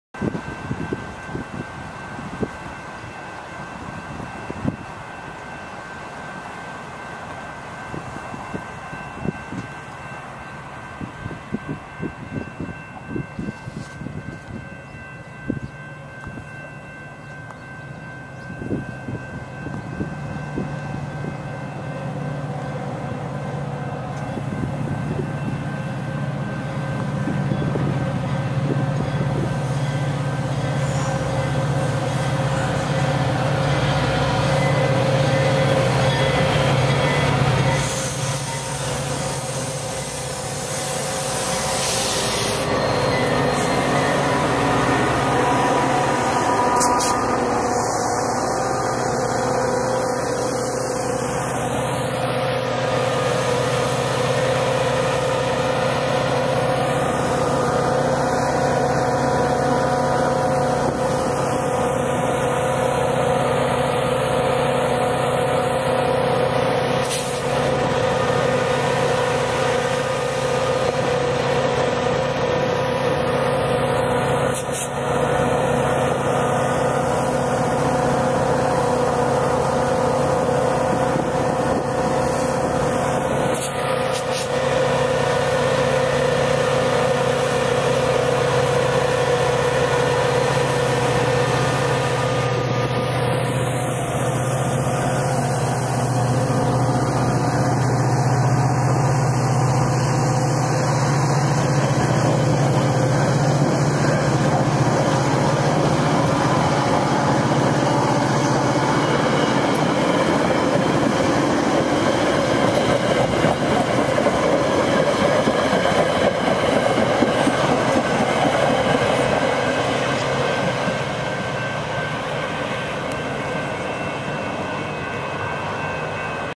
Metra EMD F40PH-3KFSB 121 MUPN 811 Wilmette
Metra EMD F40PH-3kfsb 121 brings Metra Union Pacific 811 into Wilmette with Pullman Standard coach 7848 nee 311 Chicago and North Western On home Rails on the Union Pacific north Line On the 400 and Route of the Streamliners on the Chicago and North Western Recorded on Feb  11, 2012 using a Flip Video camera.